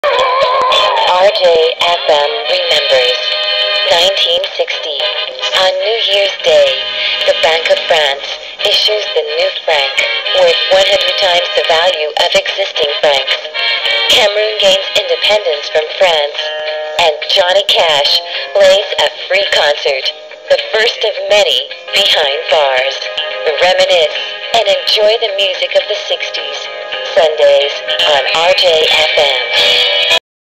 REMEMBERS THE 1960's Stage 11
New Year's Day:

January 1st: Bank of France issues new franc, worth 100 times the value of existing francs

Cameroon (French Cameroon) gains independence from France

Johnny Cash plays first of many free concerts behind bars